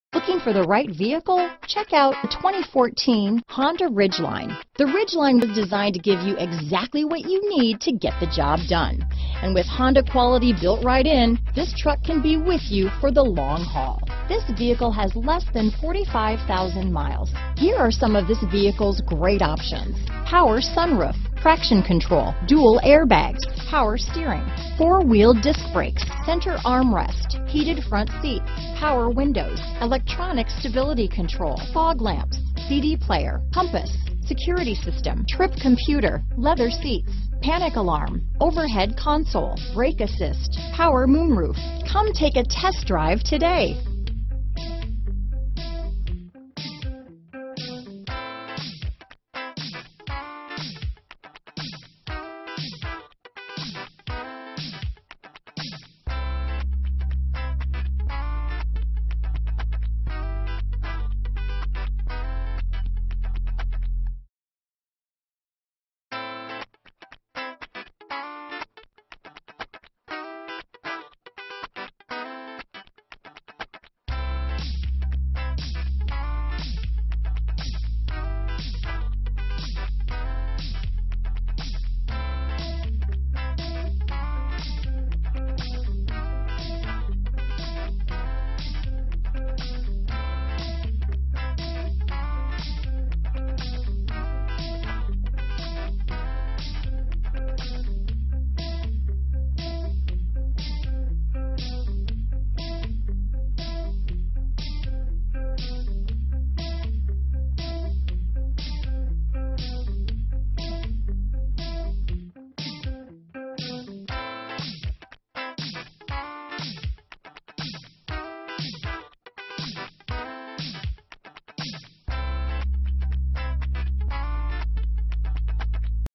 2014 Honda Ridgeline London, Springfield, Columbus, Dayton, Hilliard, OH 19T225A
Dark Cherry Pearl Used 2014 Honda Ridgeline available in London, Ohio at Buckeye Ford. Servicing the Springfield, Columbus, Dayton, Hilliard, OH area.

Buckeye Ford
110 Us Highway 42

Priced below KBB Fair Purchase Price!